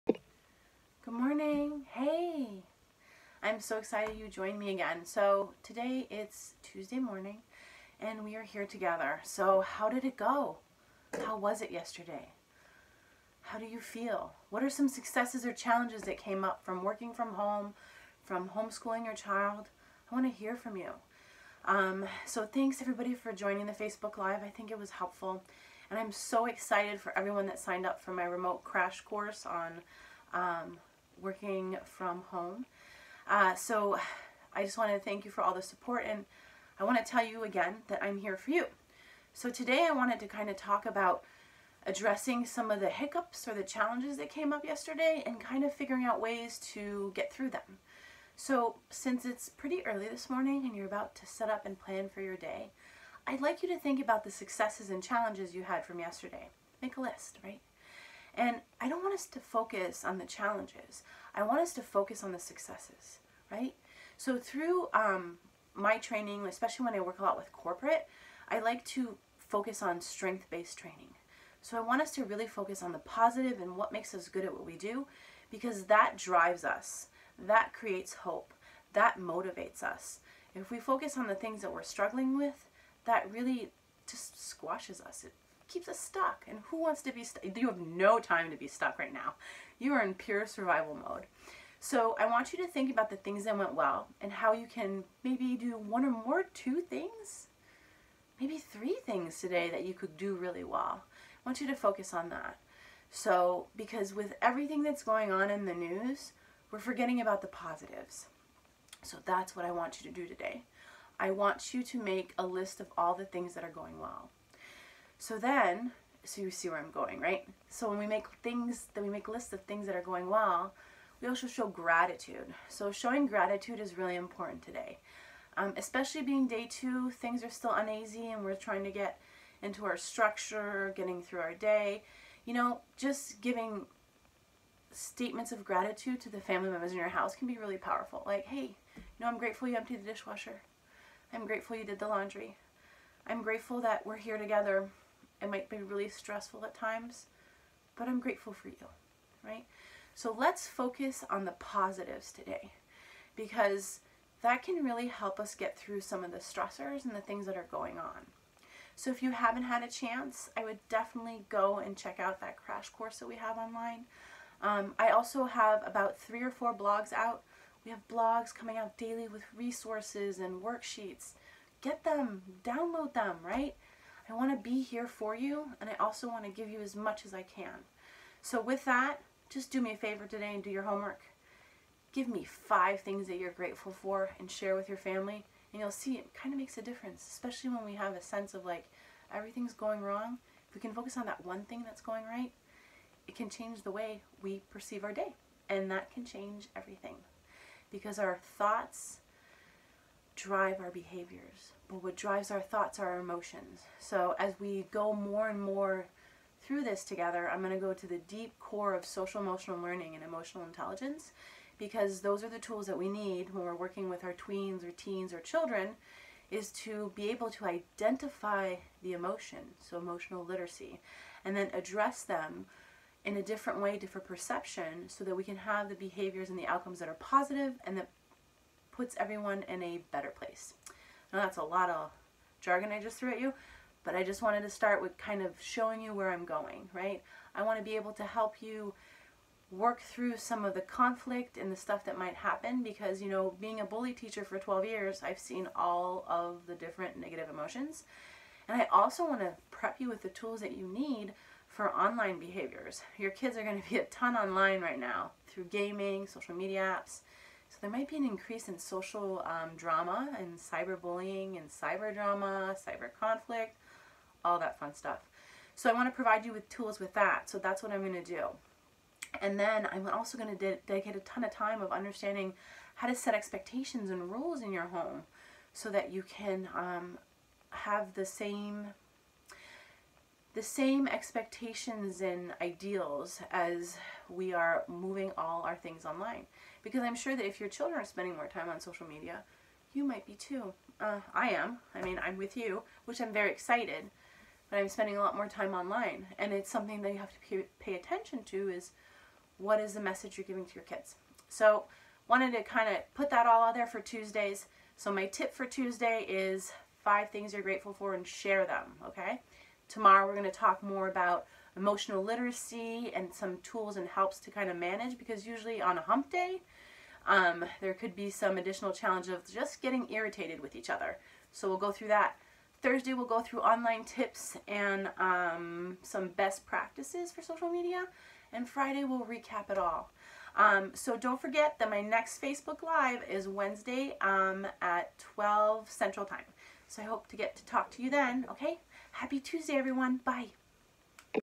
Day 4: Quarantine Parenting
Today we talk about how to focus on small successes and find the little wins. We explore ways to share gratitude with our family even when we might be getting on each other’s nerves.